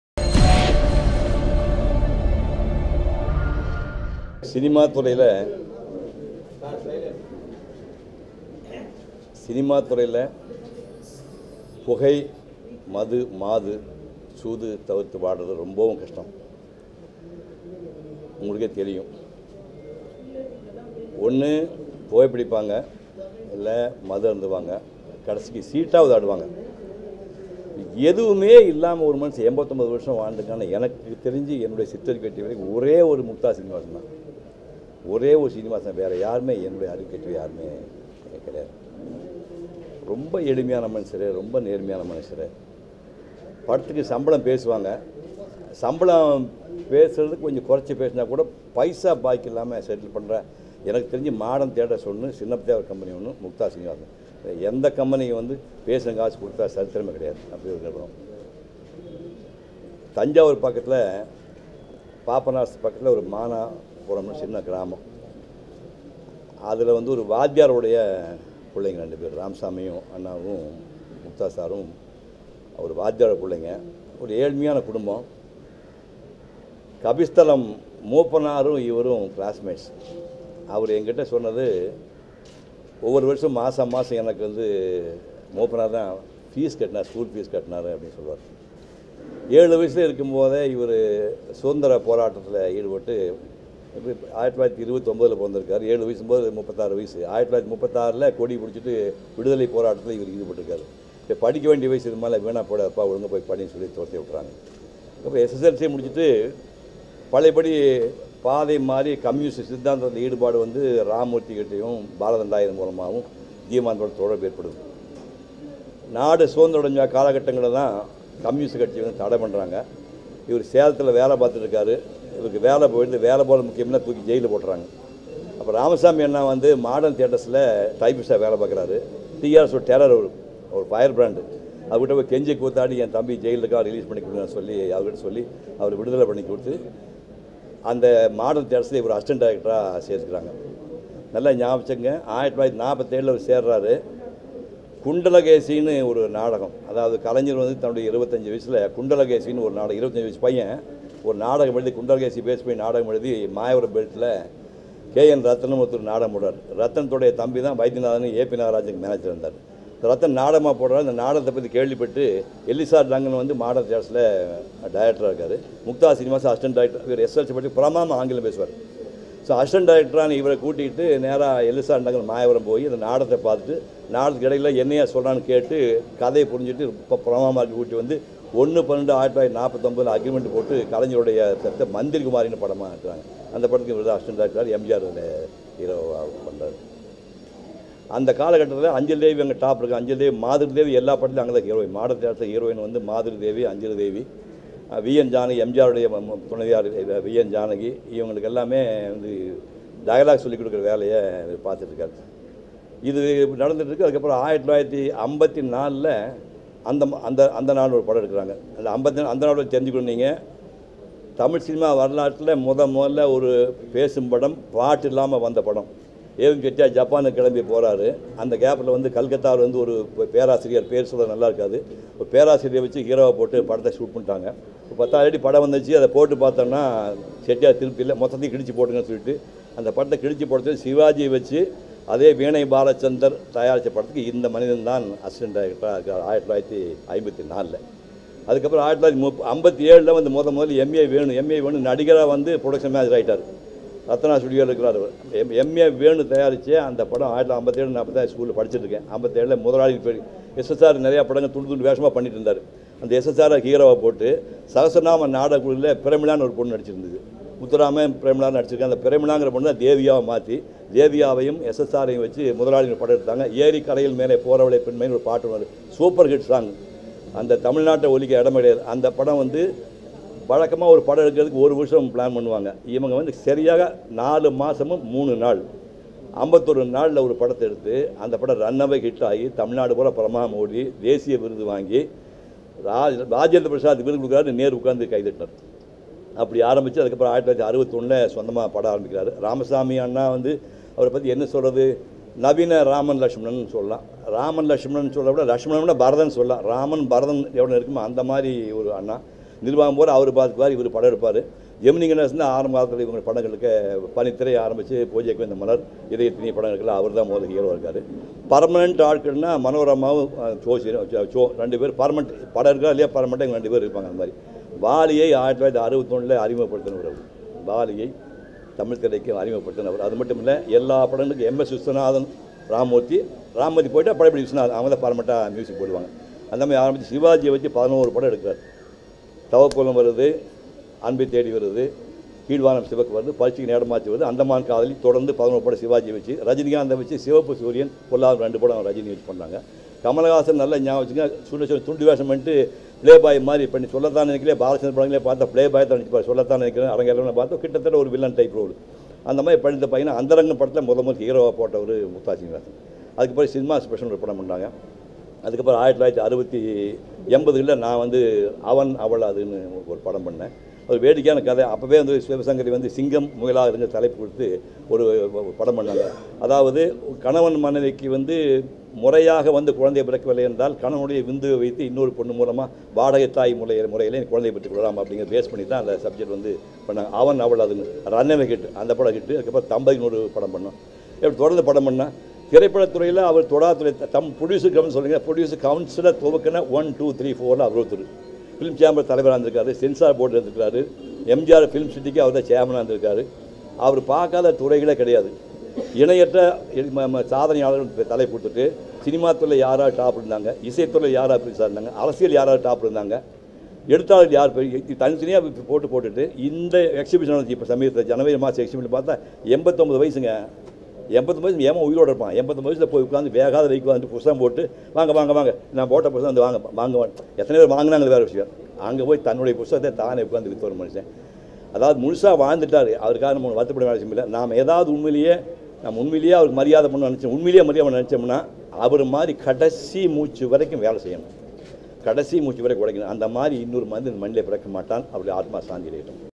பழம்பெரும் இயக்குநா் முக்தா சீனிவாசன் காலமானாா்
மது, மாது, சூது இது எதுவும் இல்லாதவர் முக்தா ஸ்ரீனிவாசன் தான் என்கிறார் சிவகுமார்